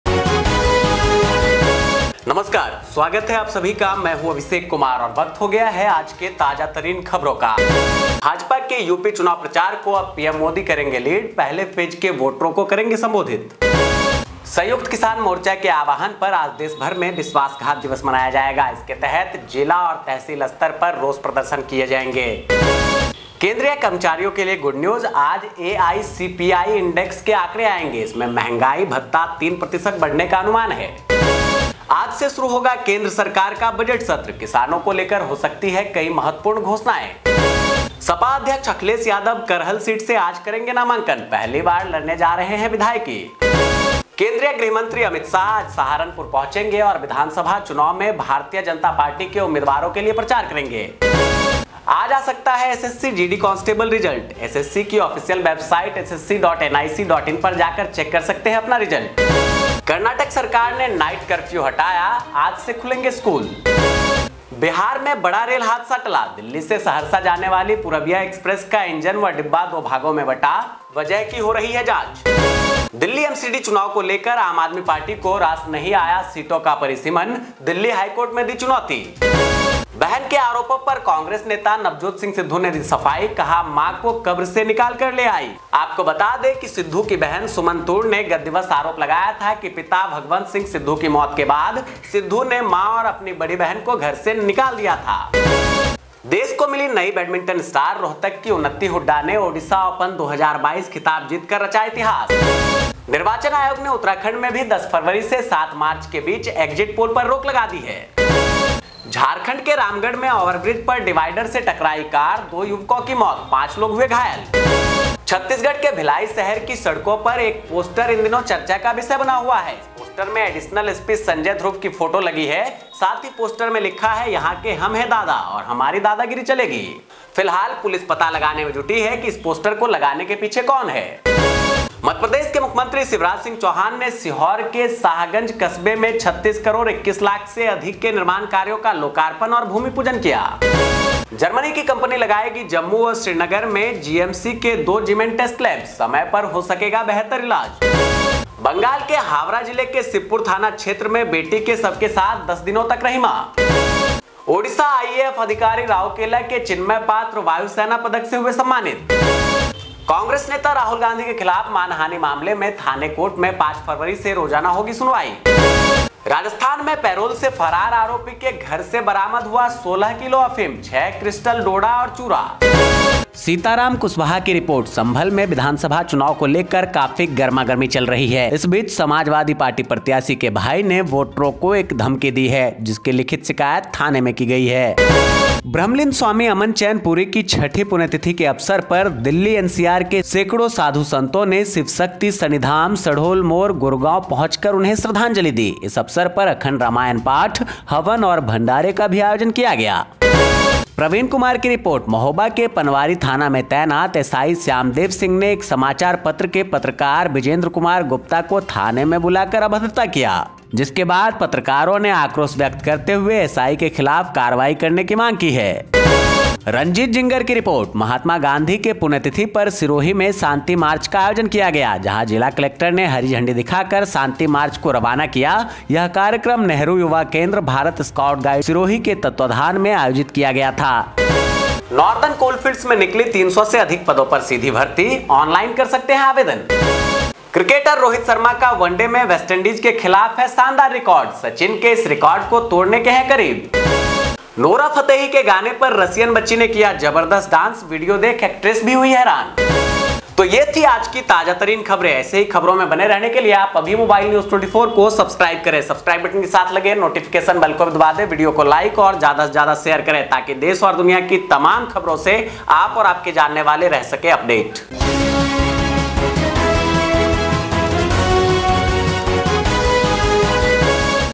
आज की ताज़ा खबरें, Aaj ki taza khabren, Mukhya samachar, News hindi me, Khas khabren, Mobile News 24
PM Modi will now lead BJP's UP election campaign, will address the voters of the first phase

Calls of United Kisan Morcha Today 'Betrayal Day' will be celebrated across the country. Under this, protest demonstrations will be organized at district and tehsil level.

Good News to Central Employees! Today the data of AICPI index will come. In this dearness allowance is expected to increase by 3%.

Central government's budget session will start from today, important announcements can be made regarding farmers

SP President Akhilesh Yadav will file nomination from Karhal seat today, MLA going to fight for the first time

Union Home Minister Amit Shah will reach Saharanpur today and campaign for Bharatiya Janata Party candidates in the assembly elections.

SSC GD constable result can come today, you can see your result on SSC official website ssc.nic.in

Karnataka government lifts night curfew, the same schools will open today

Major train accident averted in Bihar, engine and coach of Purbia Express going from Delhi to Saharsa split into two hemps, reason will be investigated

Aam Aadmi Party did not like the decision of delimitation of seats regarding Delhi MCD elections, challenged in Delhi High Court

Navjot Singh Sidhu's cleaning on the sister's allegations, brought the mother out of the grave.

The country got the new badminton star, Rohtak's Unnati Hooda created history by winning the Odisha Open 2022 title

The Election Commission has also banned exit polls in Uttarakhand between February 10 and March 7.

Car collided with divider on overbridge in Ramgarh, Jharkhand, two youths killed, five injured

A poster on the streets of Bhilai city in Chhattisgarh has become a topic of discussion these days. Additional SP Sanjay Dhruv's photo is in this poster. Also in the poster it is written, 'Here we are Dada and here we will do Dadagiri'. At present, the police is trying to find out who is behind this.